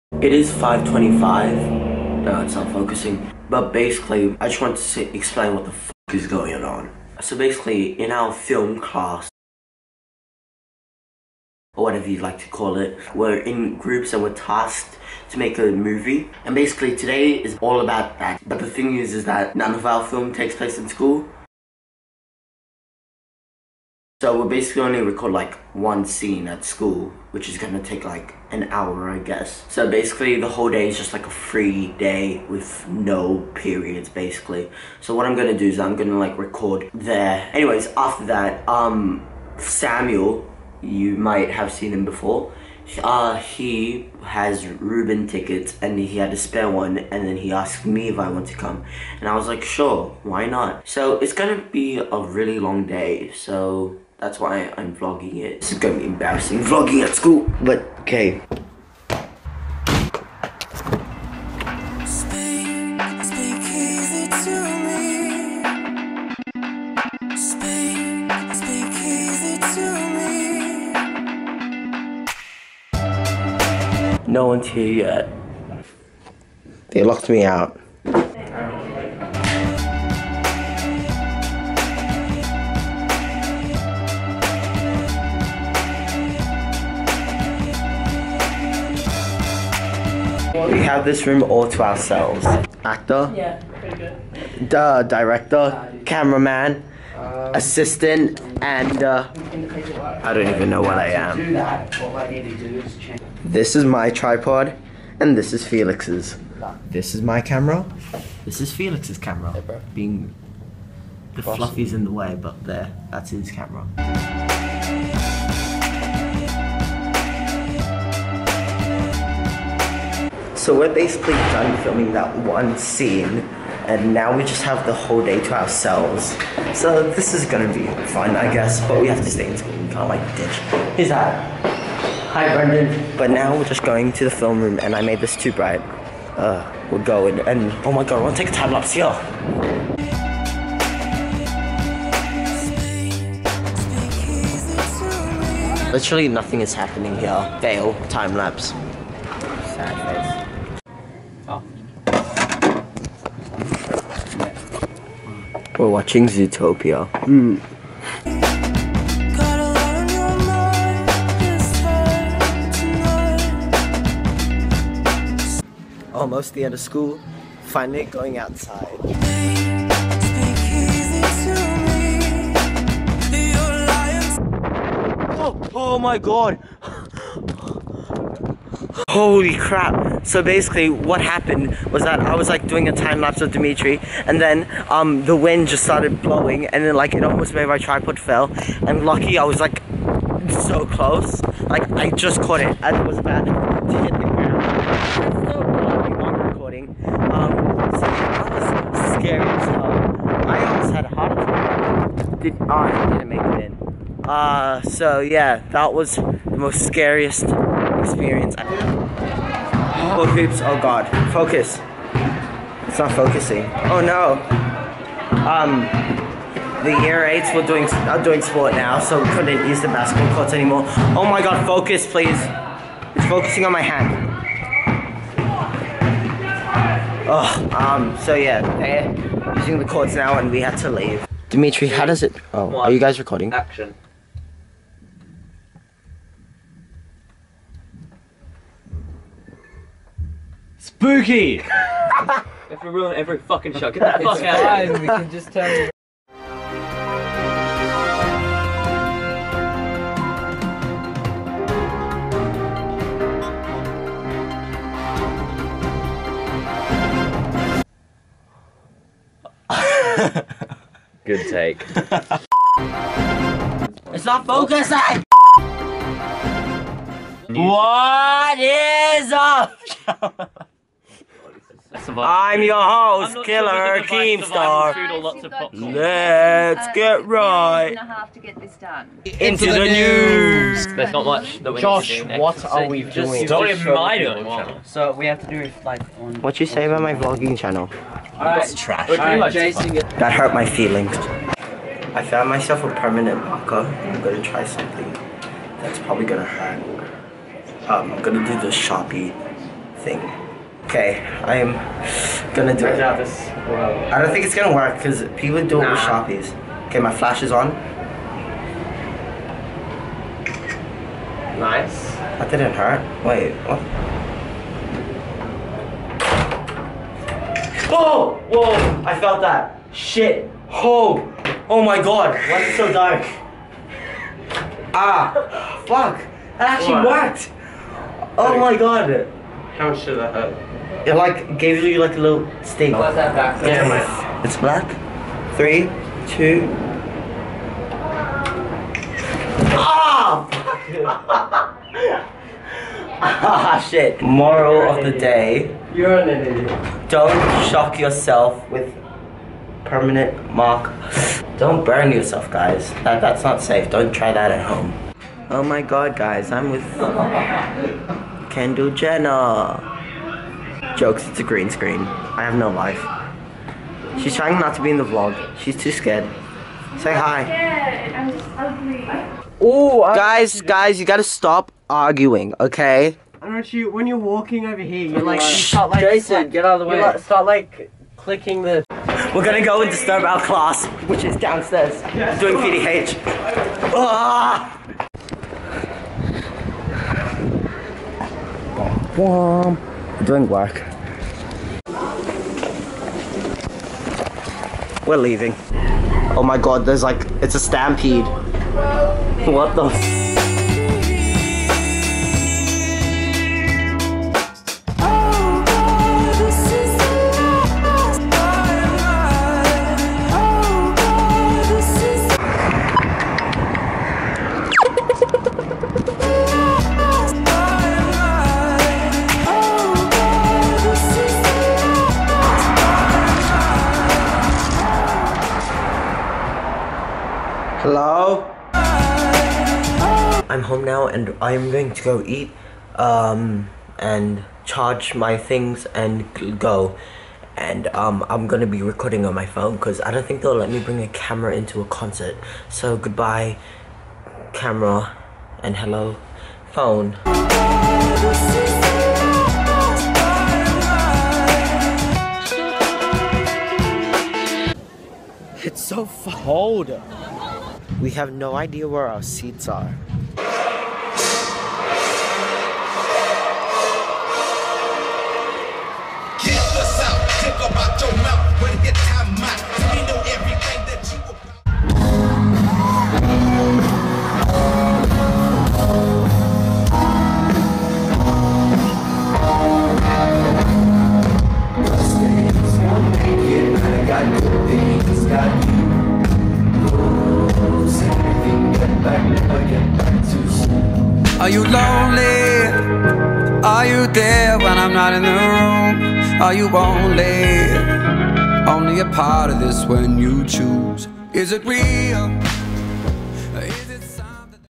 VLOGGING AT SCHOOL
eh meme
Basically me vlogging @ school cause it was film day and The Rubens concert.
vlog